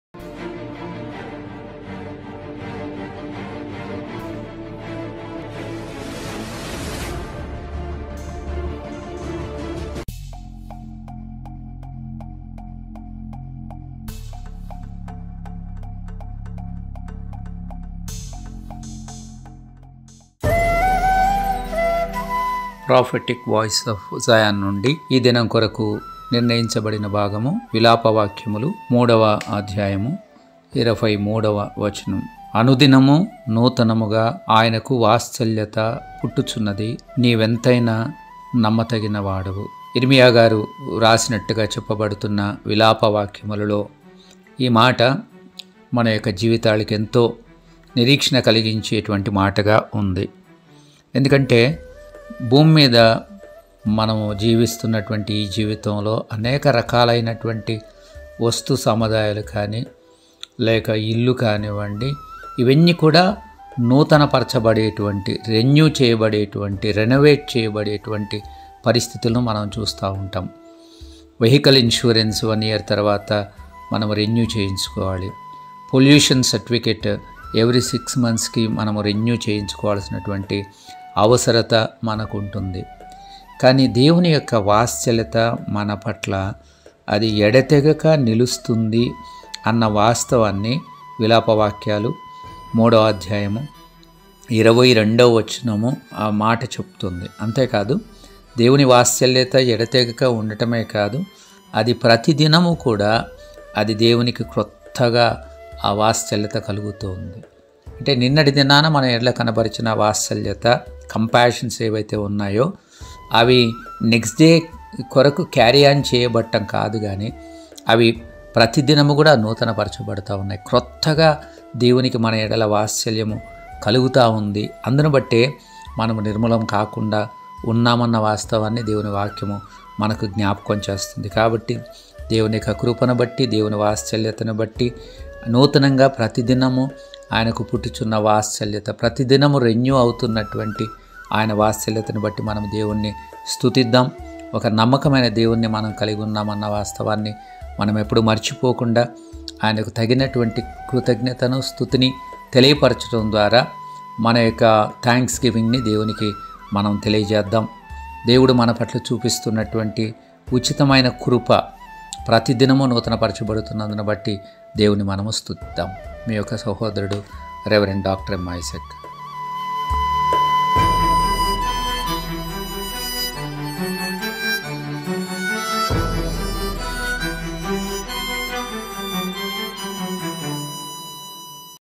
ప్రతి దినము నూతన వాత్సల్యత!!! II (11.11.22) II by Rev. Dr.M. Isaac II Zion Worship Centre II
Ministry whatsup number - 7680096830